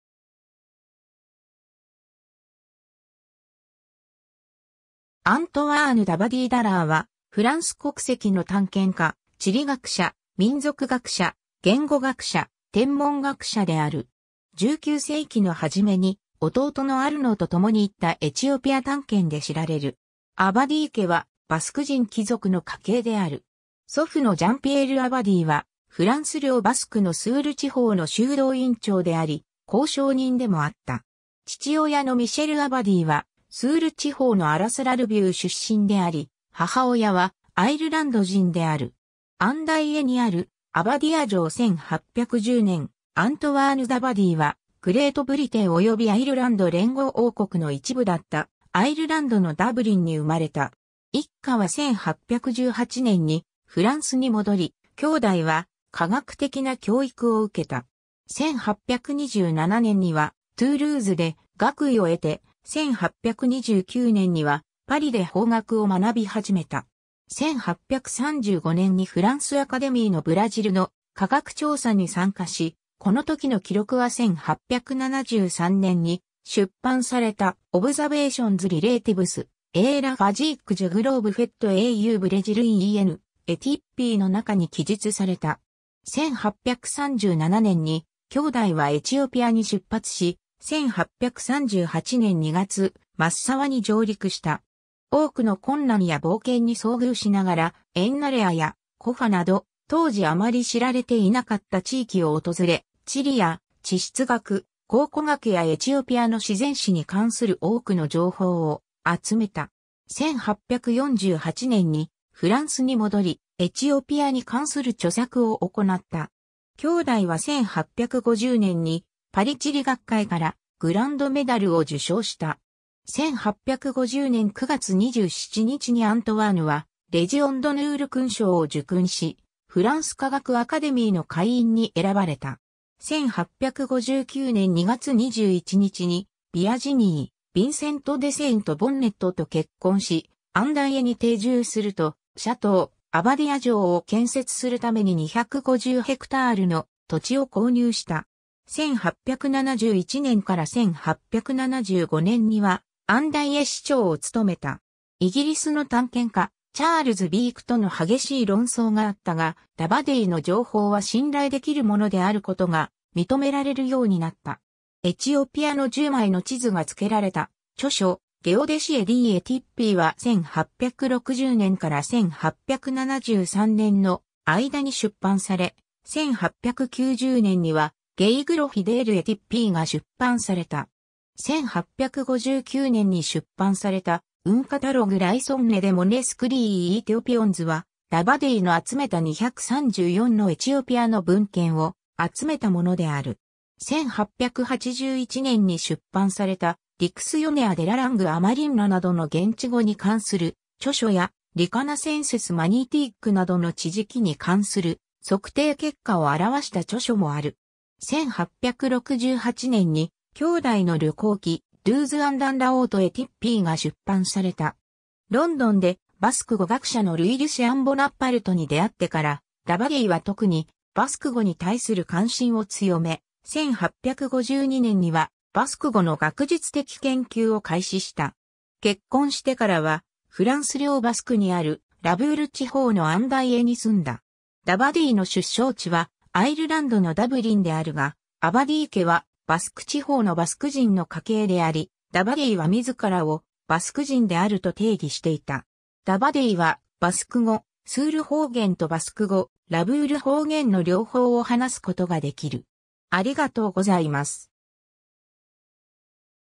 アントワーヌ・ダバディー・ダラー
アントワーヌ・ダバディー・ダラー（フランス語: Antoine Thomson d'Abbadie d'Arrast、1810年1月3日 - 1897年3月19日）は、フランス国籍の探検家・地理学者・民族学者・言語学者（バスク語）・天文学者である。
19世紀の始めに弟のアルノーとともに行ったエチオピア探検で知られる。
 アバディー家はバスク人貴族の家系である。
祖父のジャン＝ピエール・アバディーはフランス領バスクのスール地方の修道院長であり公証人でもあった。
父親のミシェル・アバディーはスール地方のアラスト＝ラルビュー出身であり、母親はアイルランド人である。
 アンダイエにあるアバディア城 1810年、アントワーヌ・ダバディーはグレートブリテン及びアイルランド連合王国の一部だったアイルランドのダブリンに生まれた。
一家は1818年にフランスに戻り、兄弟は科学的な教育を受けた。
1827年にはトゥールーズで学位を得て、1829年にはパリで法学を学び始めた。
 1835年にフランス・アカデミーのブラジルの科学調査に参加し、この時の記録は1873年に出版された『Observations relatives a la physique du globe faites au Bresil et en Ethiopie』の中に記述された。
1837年に兄弟はエチオピアに出発し、1838年2月マッサワに上陸した。
多くの困難や冒険に遭遇しながらEnnareaやKaffaなど当時あまり知られていなかった地域を訪れ、地理や地質学、考古学やエチオピアの自然史に関する多くの情報を集めた。
1848年にフランスに戻り、エチオピアに関する著作を行った。
 兄弟は1850年にパリ地理学会からグランドメダルを受賞した。
1850年9月27日にアントワーヌはレジオンドヌール勲章を受勲し、フランス科学アカデミーの会員に選ばれた。
1859年2月21日にVirginie Vincent de Saint-Bonnetと結婚し、アンダイエに定住するとシャトー「アバディア城」を建設するために250ヘクタールの土地を購入した。
1871年から1875年にはアンダイエ市長を務めた。
 イギリスの探検家、チャールズ・ビークとの激しい論争があったが、ダバディーの情報は信頼できるものであることが認められるようになった。
エチオピアの10枚の地図が付けられた著書『Geodesie d'Ethiopie』（エチオピアの測地学）は1860年から1873年の間に出版され、1890年には『Geographie de l'Ethiopie』（エチオピアの地理）が出版された。
1859年に出版された『Un Catalogue raisonne de manuscrits ethiopiens』はダバディーの集めた234のエチオピアの文献を集めたものである。
1881年に出版された『Dictionnaire de la langue amarinna』などの現地語に関する著書や、『Reconnaissances magnetiques』などの地磁気に関する測定結果を著した著書もある。
1868年に兄弟の旅行記『Douze ans dans la Haute Ethiopie』が出版された。
 ロンドンでバスク語学者のルイ・リュシアン・ボナパルトに出会ってから、ダバディーは特にバスク語に対する関心を強め、1852年にはバスク語の学術的研究を開始した。
結婚してからはフランス領バスクにあるラブール地方のアンダイエに住んだ。
ダバディーの出生地はアイルランドのダブリンであるが、アバディー家はバスク地方のバスク人の家系であり、ダバディーは自らをバスク人であると定義していた。
ダバディーはバスク語スール方言とバスク語ラブール方言の両方を話すことができる。